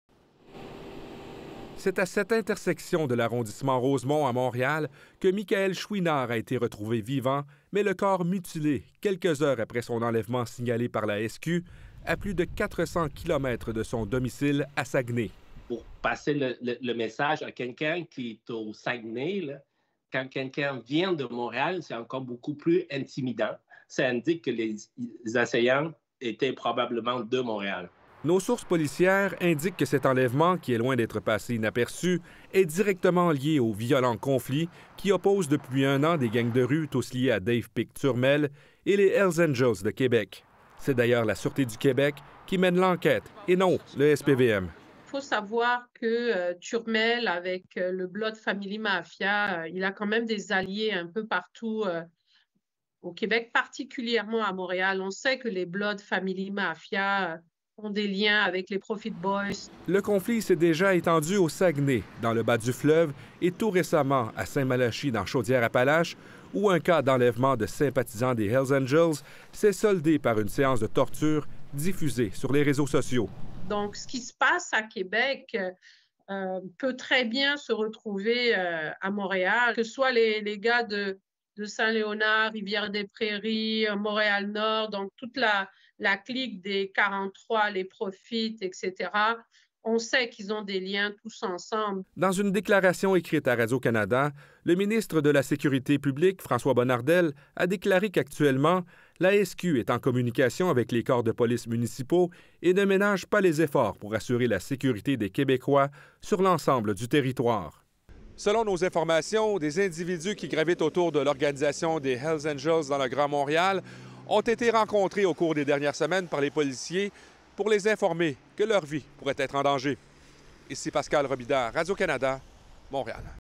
Guerre de trafiquants : un proche des motards enlevé et mutilé
L’homme de Saguenay porté disparu depuis mercredi a été retrouvé sur le trottoir d’une rue de l’arrondissement Rosemont, à Montréal. Michaël Chouinard est vivant, mais il a été gravement mutilé. On lui a coupé un orteil et un doigt et il portait des blessures un peu partout sur le corps.

Le reportage de Pascal Robidas.